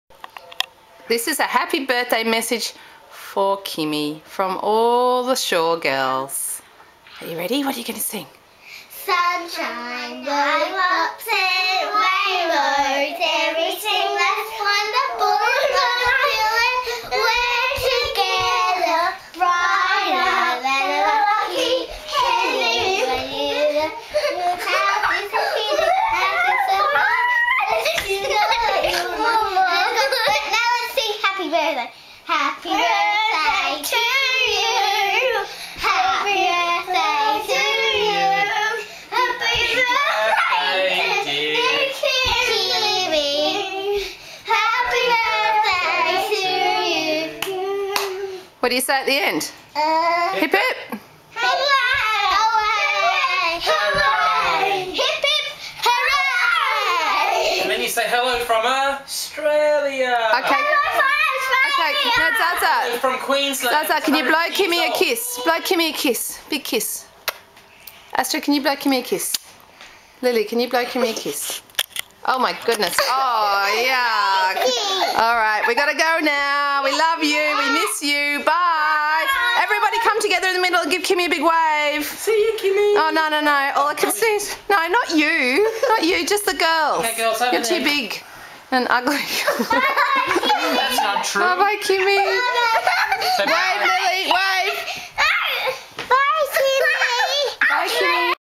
Happy Birthday Kimi 2009
Astrid, Lily and Zara (and Marni and James) made a movie to wish Kimi a Happy Birthday. The file however ended up being a 170mg .avi file and would not load on Youtube. Marni finally got her act together 3 months after Kimi's birthday, worked out how to turn it into a 3mg Quicktime file and voila - a Youtube victory, and a belated birthday message finally is delivered to it's much loved and missed recipient. xoxoxoxo